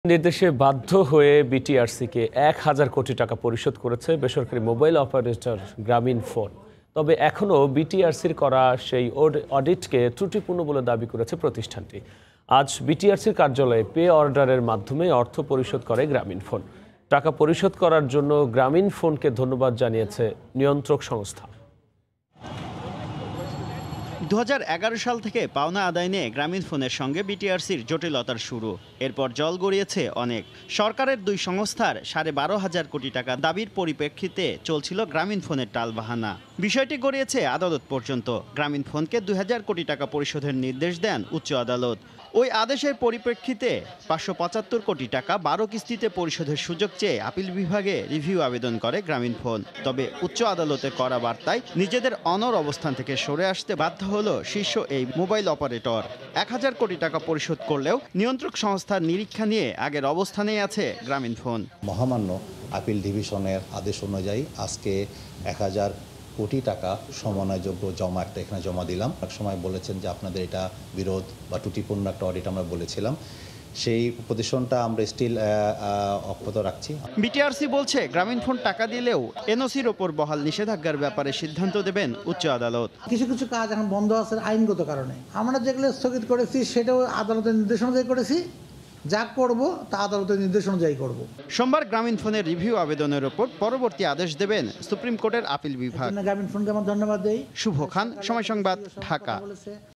অবশেষে বিটিআরসিকে এক হাজার কোটি টাকা পরিশোধ করলো গ্রামীণফোন | GP | BRTC
উচ্চ আদালতের নির্দেশে, বাধ্য হয়ে বিটিআরসিকে এক হাজার কোটি টাকা পরিশোধ করেছে বেসরকারি মোবাইল অপারেটর গ্রামীণফোন। তবে, এখনো বিটিআরসির করা অডিটকে ত্রুটিপূর্ণ বলে দাবি করেছে প্রতিষ্ঠানটি। আজ বিটিআরসির কার্যালয়ে পে-অর্ডারের মাধ্যমে এ অর্থ পরিশোধ করে গ্রামীণফোন। টাকা পরিশোধ করায় জন্য গ্রামীণফোনকে ধন্যবাদ জানিয়েছে নিয়ন্ত্রক সংস্থা।